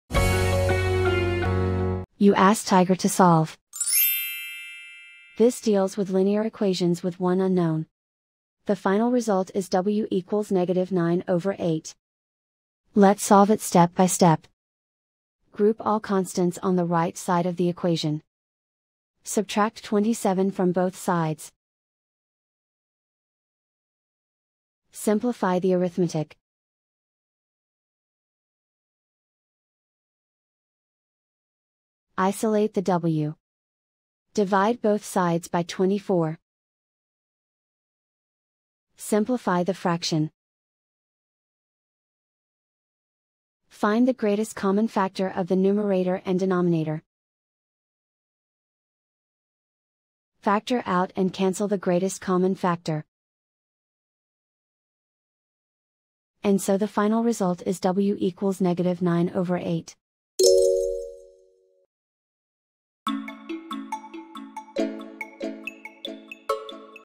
Linear equation with one unknown: Solve 24w+27=0 step-by-step solution
Solving 24w+27=0 using the linear equation with one unknown solver

for a step-by-step and more tips and tricks for dealing with linear equations with one unknown.